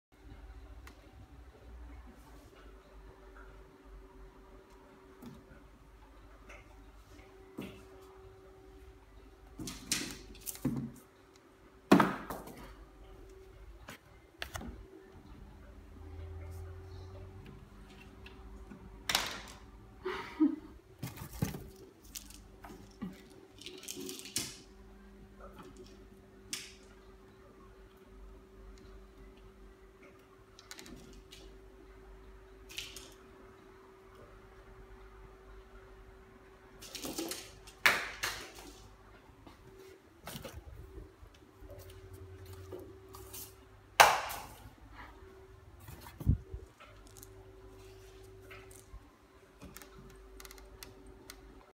Knocking stuff down
Felippo wants to keep  the shelf clean knocking his toys down 😸